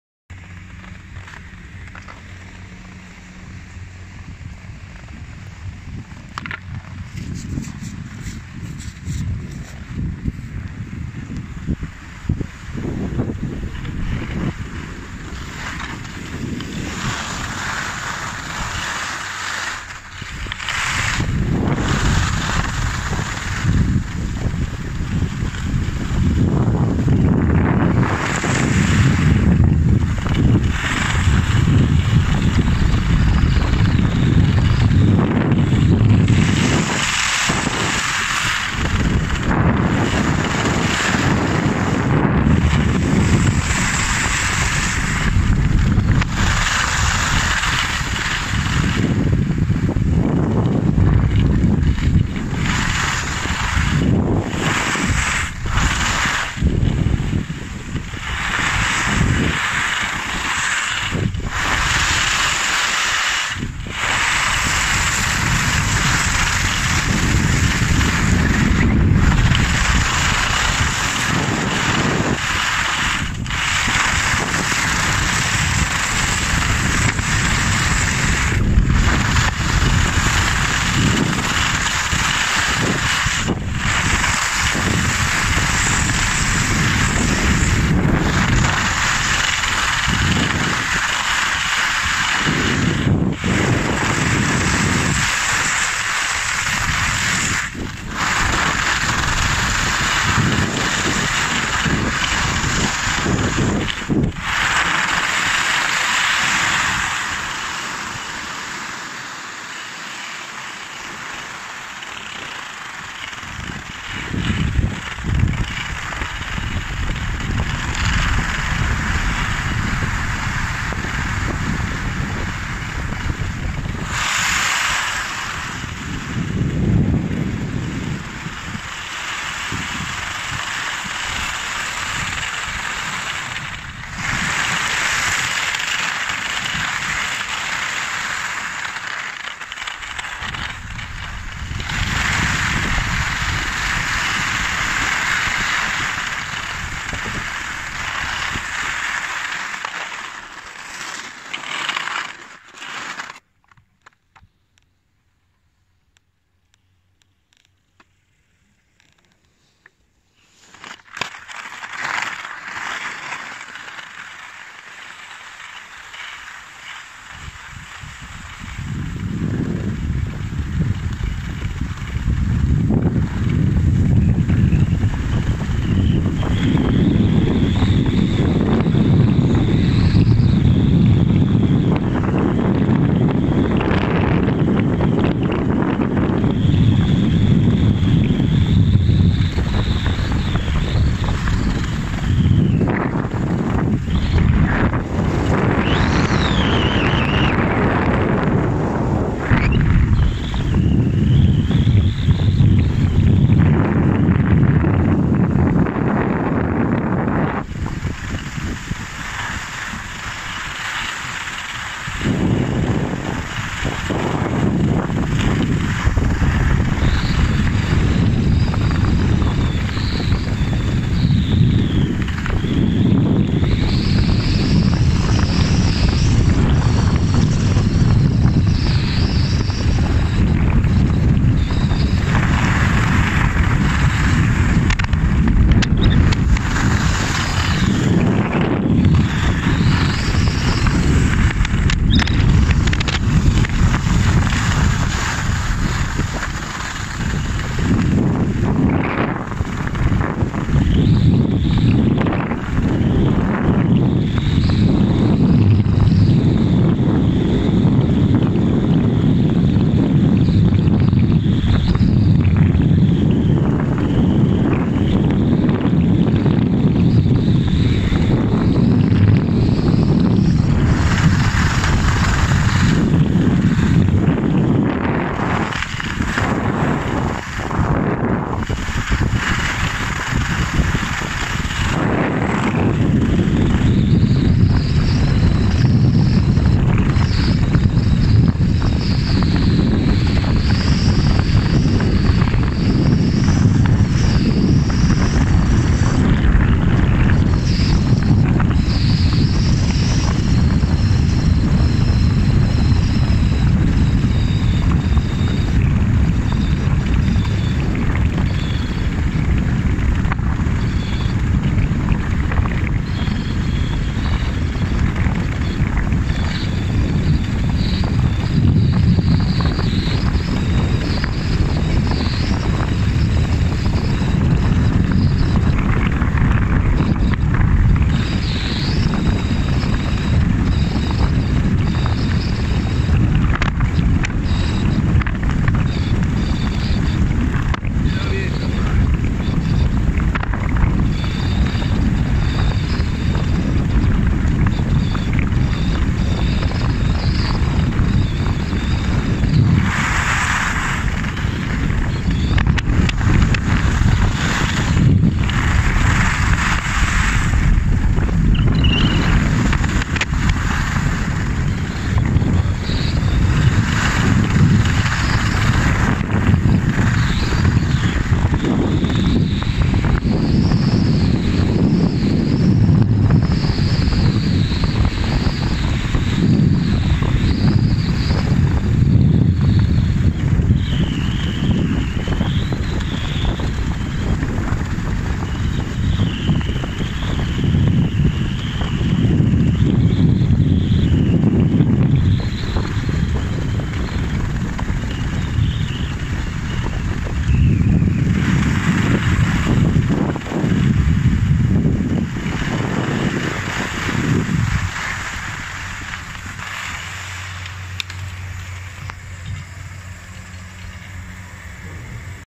Pista Valscesa al Baradello ad Aprica - Corteno Golgi
Ecco il nostro video dellaPista Valscesa che si trova al Baradello nel comprensorio Aprica - Corteno Golgi.

La si raggiunge scendendo dalla seggiovia del Baradello e stando alla sinistra. Il primo pezzo è piano ma poi si apre un bel muro classificato come rosso ma che se ghiacciato può dare problemi.

Nel video siamo poi proseguiti lungo la pista Superpanoramica, una blu molto larga e adatta a tutti che conduce in paese.